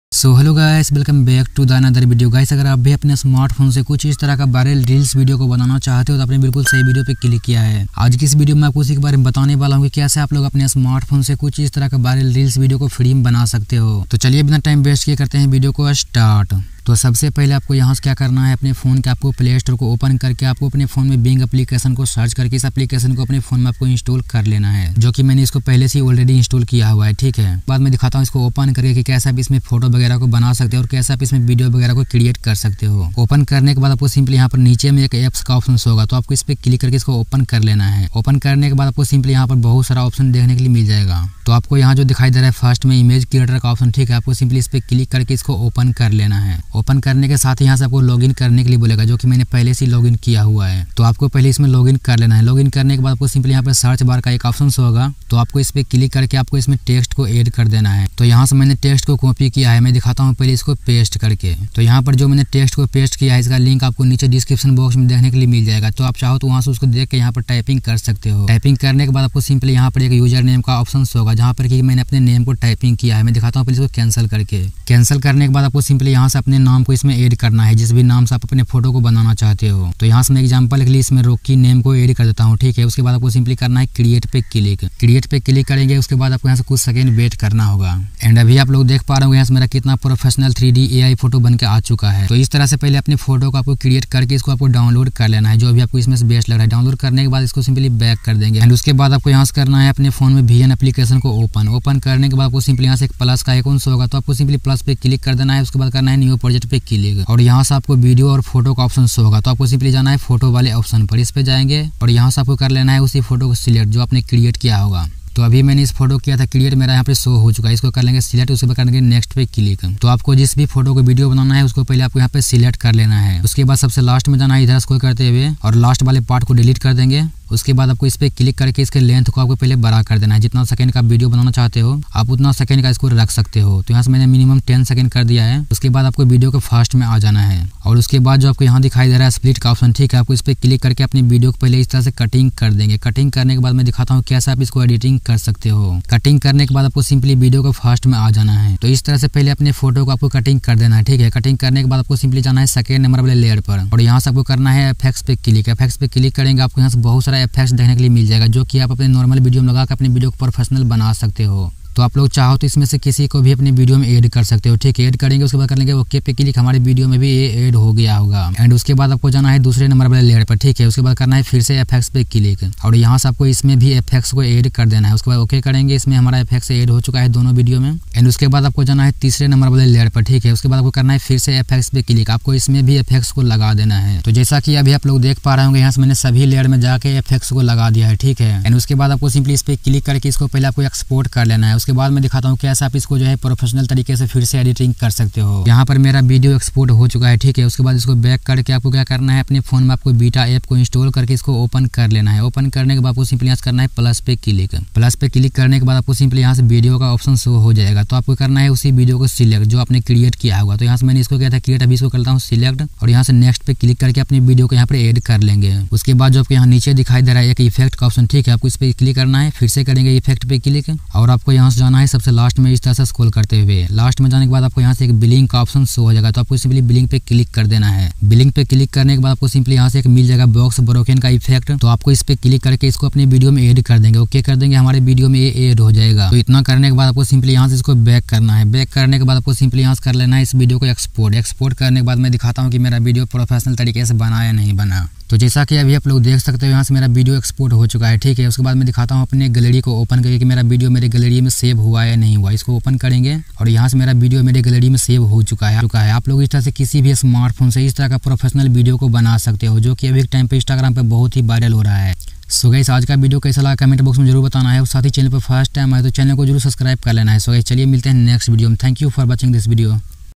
🥰 Instagram Viral Couple Name Ai Video Editing Bing Image Creator Tutorial | Couple Name Video Edit
∆ Prompts ∆

Create a 3D illusion for a WhatsApp profile picture where a boy in a black shirt sits casually on a Wingback Elefent. Wearing sneakers, a black cricket cap, and sunglasses, he looks ahead. The background features "Rocky" in big and capital white fonts on the black wall. There should not be his shadow, and there are wings to make it appear as if he is an angel.

                            ⚡Your Queries ⚡